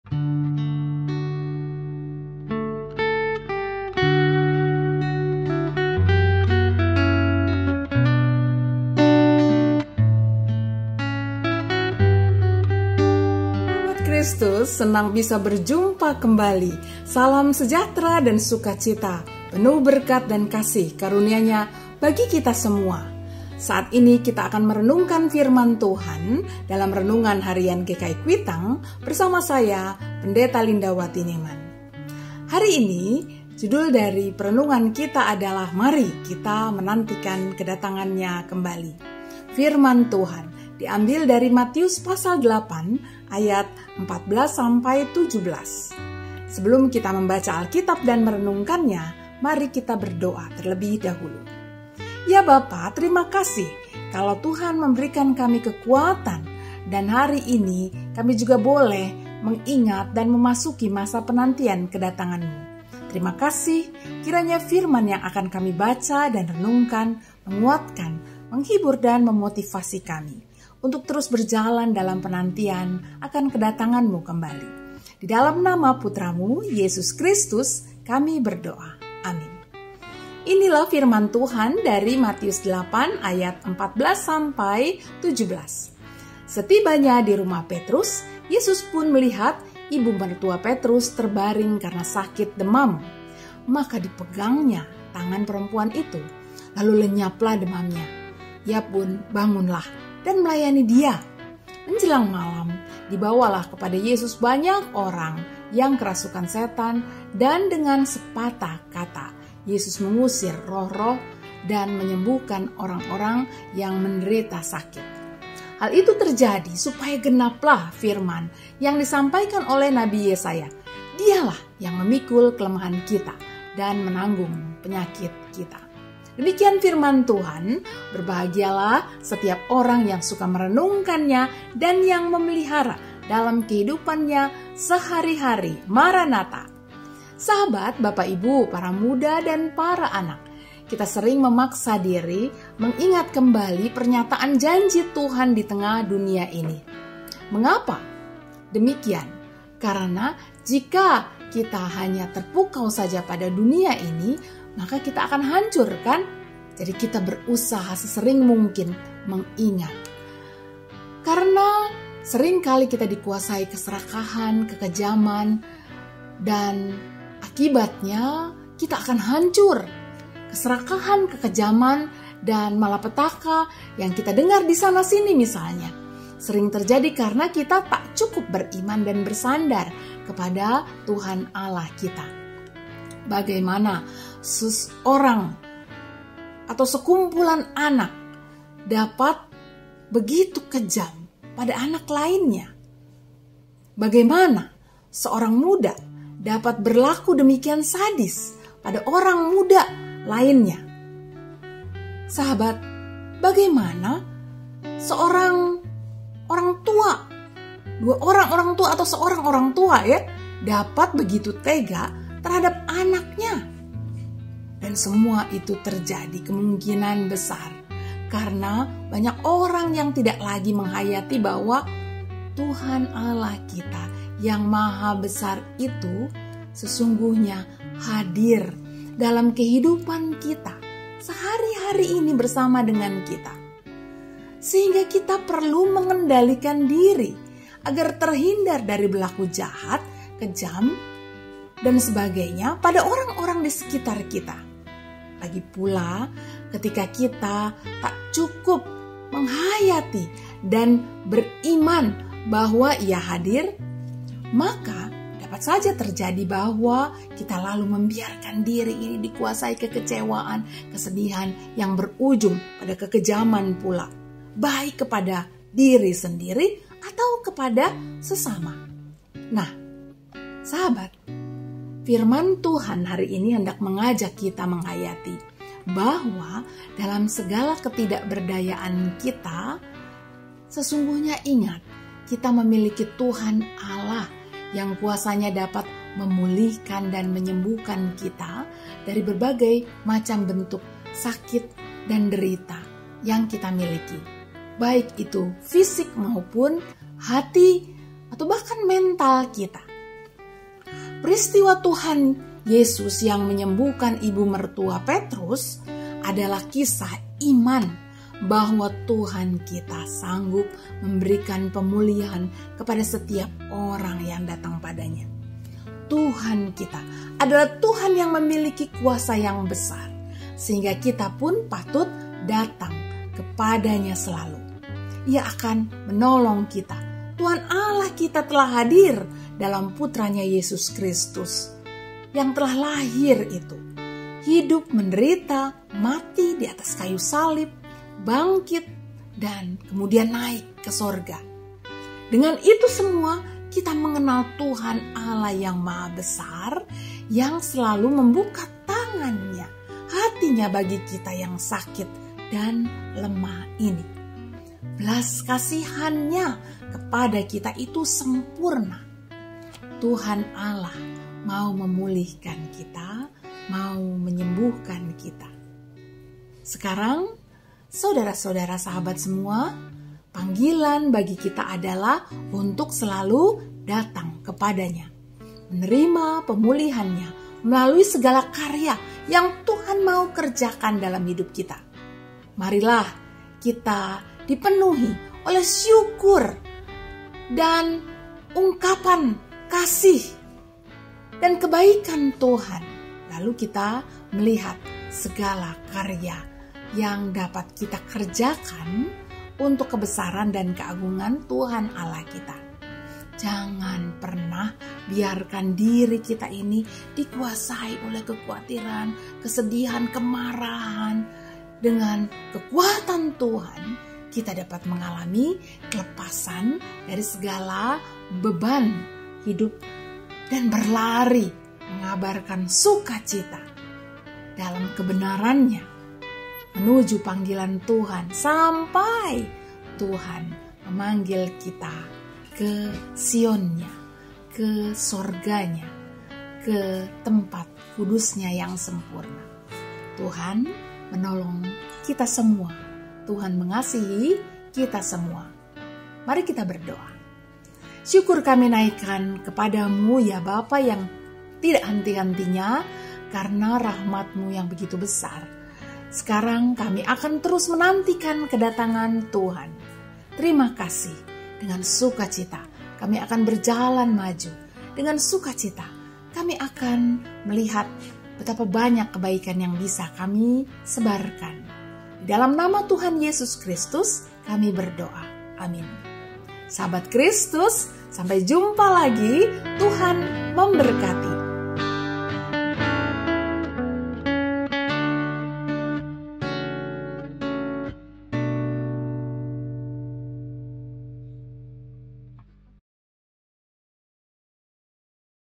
Renungan Harian GKI Kwitang, Rabu 13 Desember 2023
Bersama: Pdt. Lindawati Niman
Tema: Mari Menanti KedatanganNya Kembali
Bacaan: Matius 8:14-17